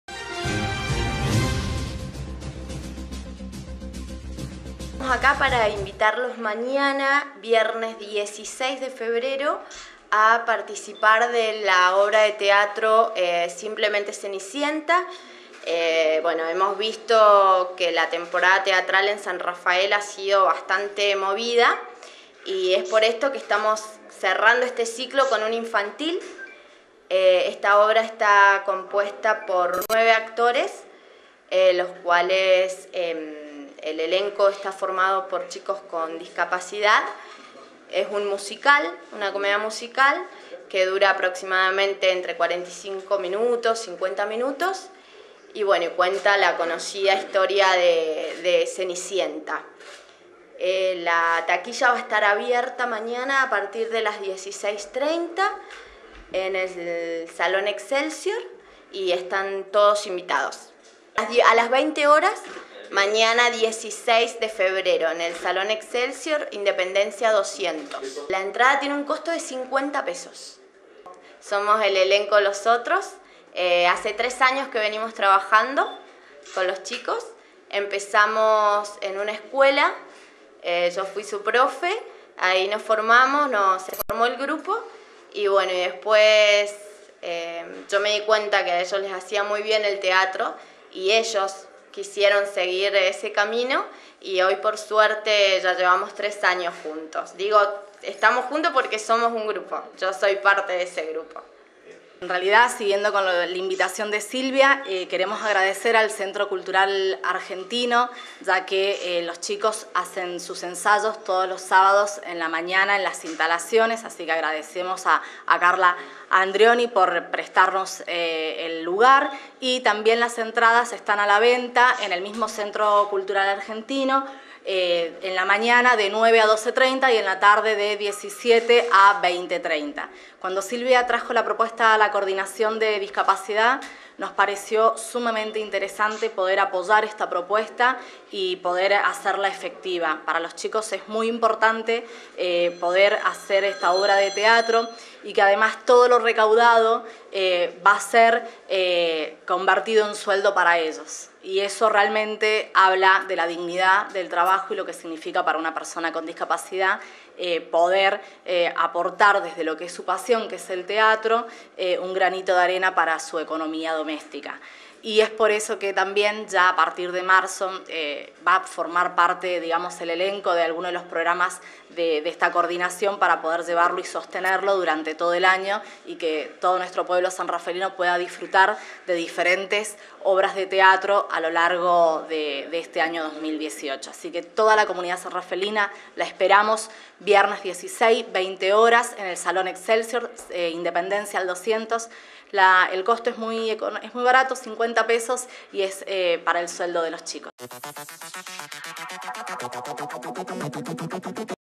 TEATRO SOLIDARIO
En la Sala Excelsior de nuestra ciudad de San Rafael,, un grupo de nueve actores con discapacidades diferentes, montan una obra para todo público, basada en el universal cuento de Caperucita Roja. Desde la Coordinación del Área de Discapacidad del Municipio apoyan la iniciativa e invitan al pueblo de San Rafael a sumarse a este emprendimiento artístico. La recaudación se convertirá en los 'sueldos' de los actores.